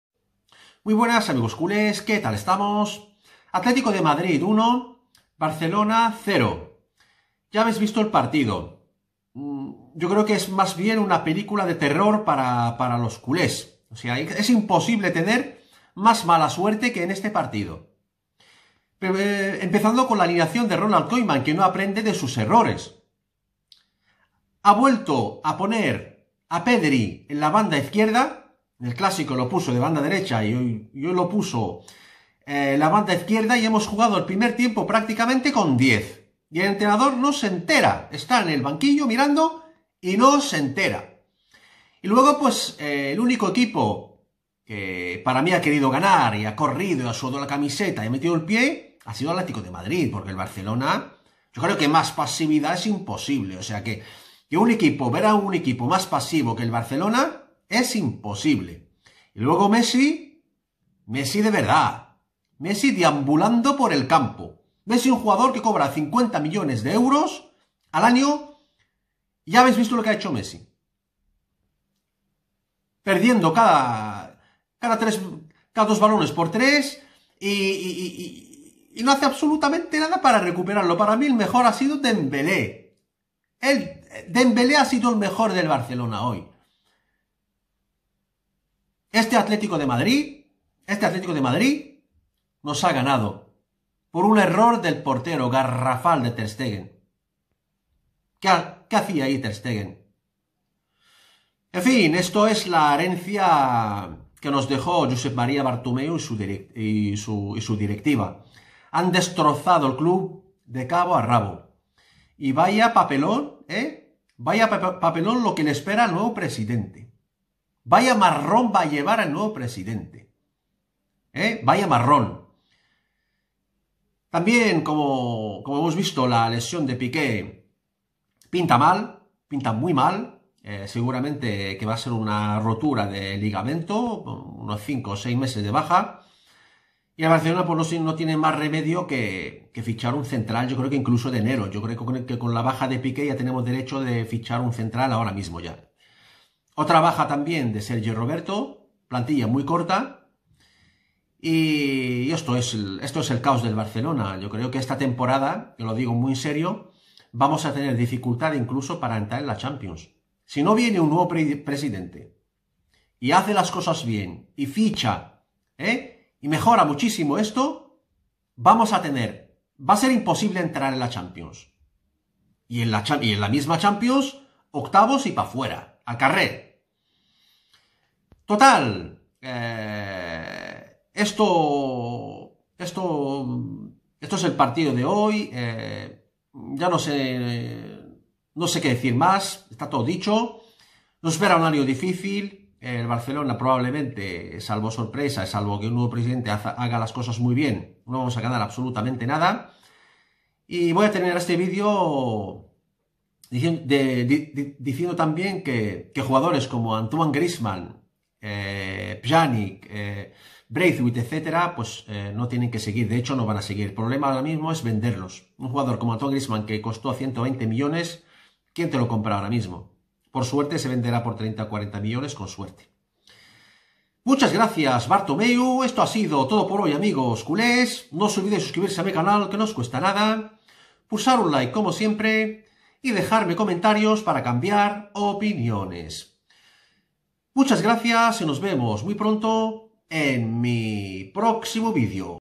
ATLÉTICO DE MADRID VS BARCELONA 1-0 | RESUMEN DEL PARTIDO | LA LIGA SANTANDER 2020/21
Un triste Barcelona pierde en el Wanda contra el Atlético de Madrid por un gol a cero, dando una muy mala imagen y perdiendo a piqué por una grave lesión